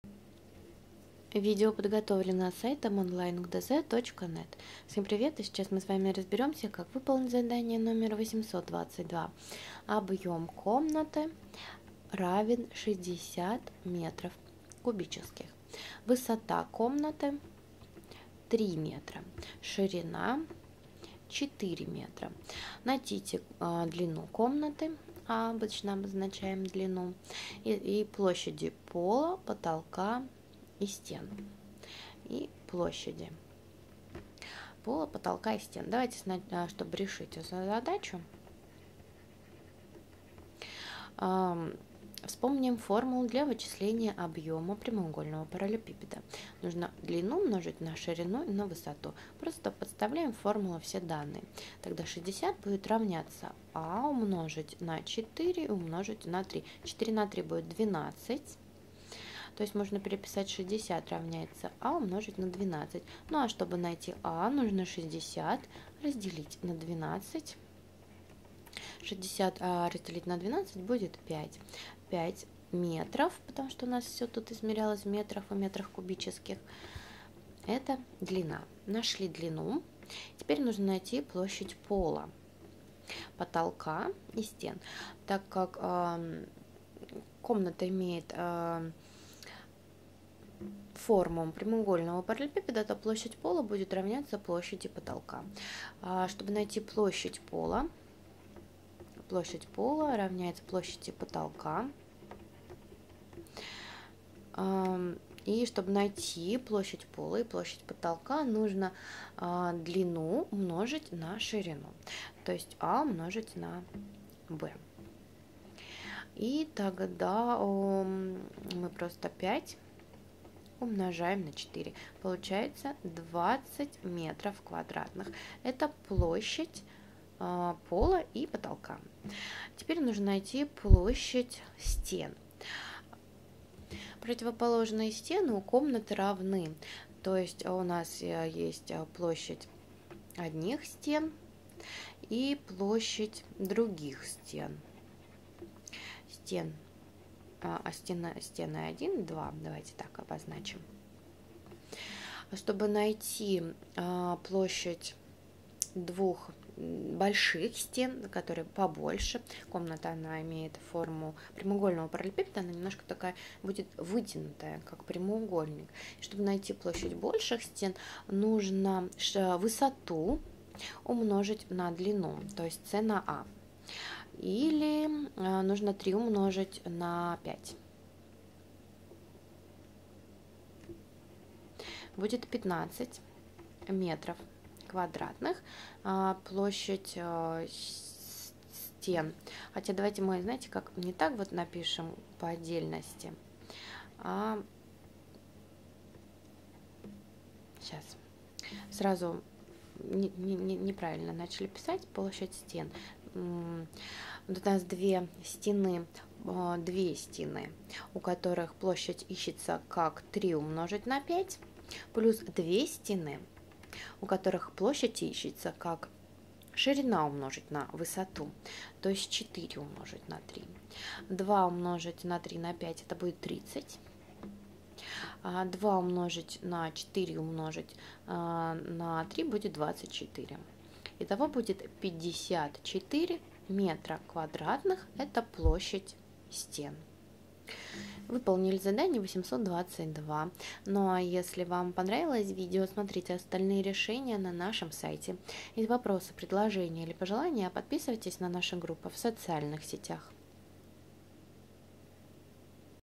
Задание № 822 - Математика 5 класс (Виленкин, Жохов)
В задании 822 вам необходимо найти длину комнаты, если известны ее объем, высота и ширина. Все очень просто — вы должны подставить имеющиеся данные в формулу для нахождения объема прямоугольного параллелепипеда и выполнить получившееся уравнение. Чтобы сделать это задание без ошибок, воспользуйтесь нашим видео с решением.